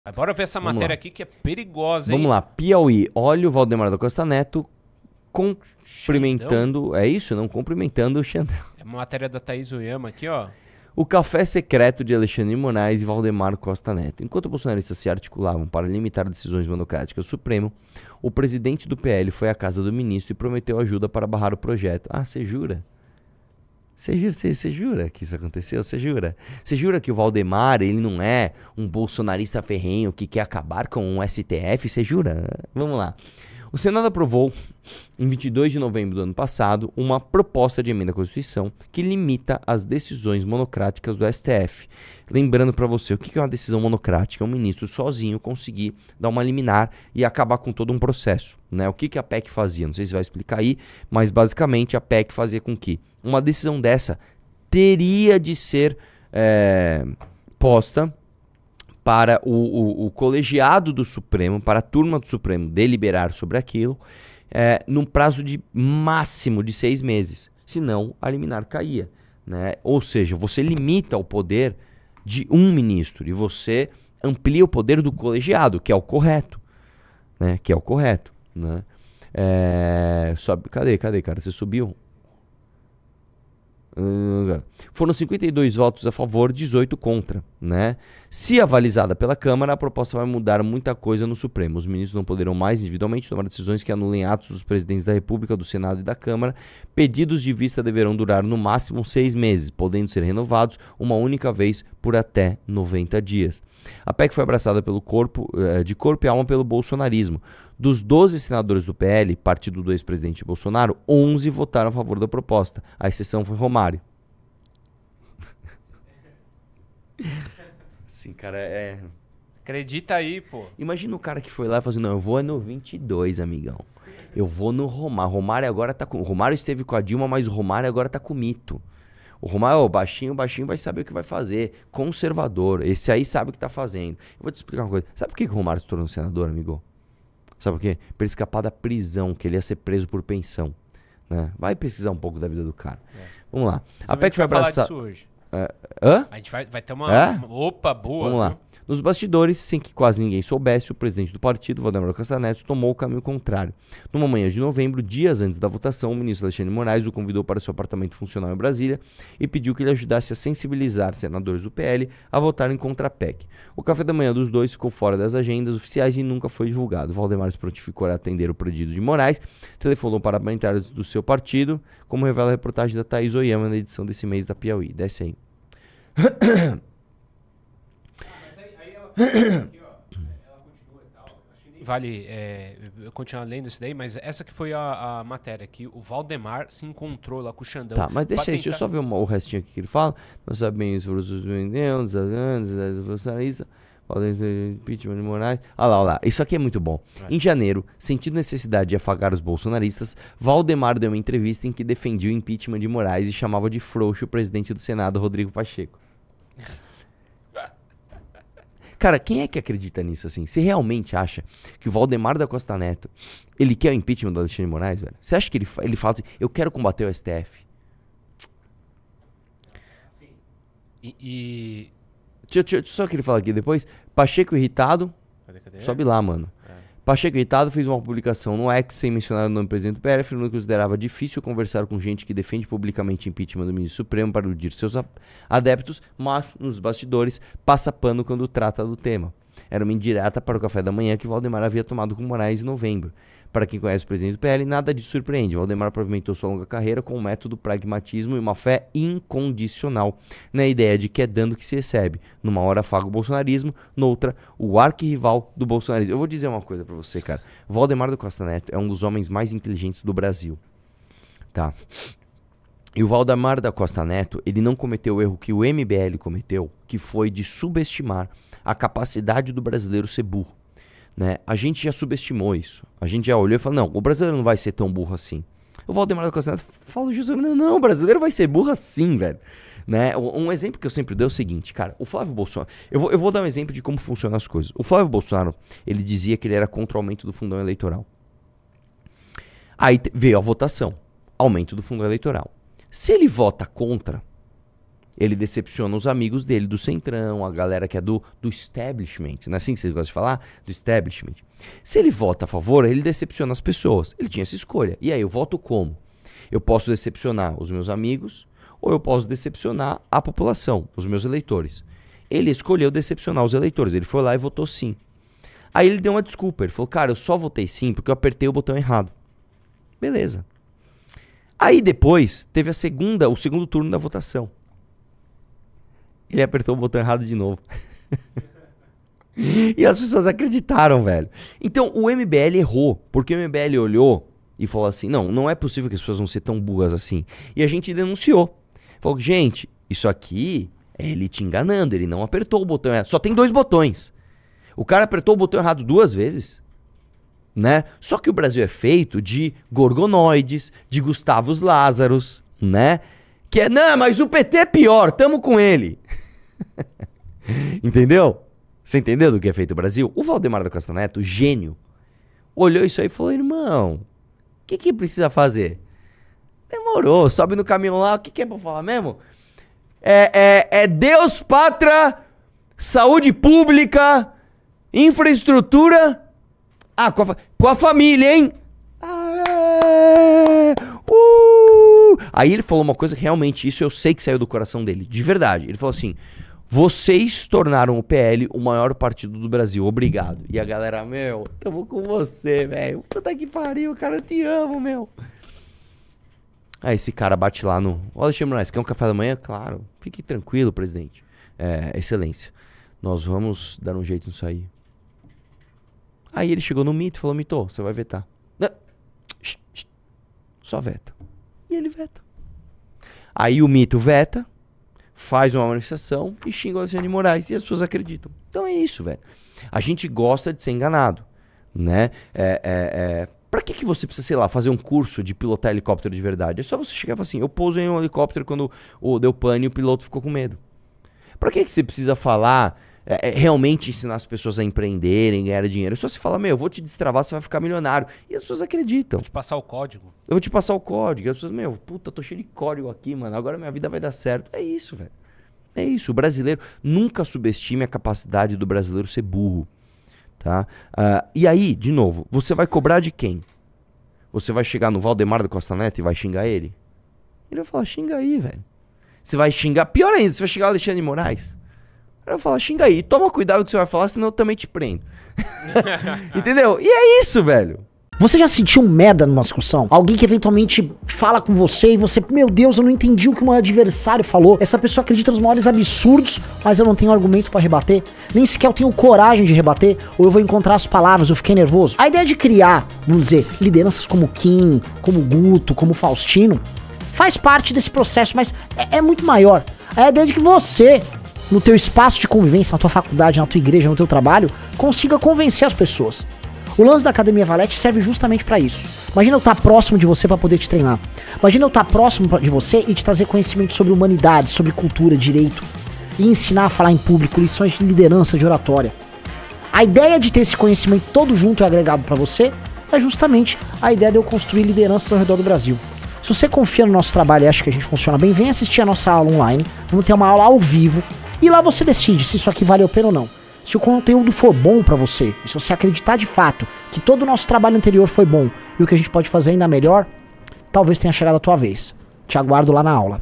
XANDÃO TOMOU CAFÉ COM DONO DO BOLSONARO! | Cortes do
O presidente do Partido Liberal (PL), Valdemar Costa Neto, e o ministro do Supremo Tribunal Federal (STF), Alexandre de Moraes, protagonizaram um encontro supostamente inusitado. Enquanto bolsonaristas se empenhavam em avançar com uma proposta que visava limitar as decisões monocráticas dos ministros do Supremo, Valdemar tentou se dirigir, discretamente, a um encontro com Xandão. Durante a reunião, que deveria ter se mantido longe dos olhos públicos, o líder do PL prometeu apoio para barrar o projeto, evidenciando uma complexa rede de alianças e negociações nos corredores do poder.

✅Ouça o MBL News também em versão podcast. São 9 plataformas disponíveis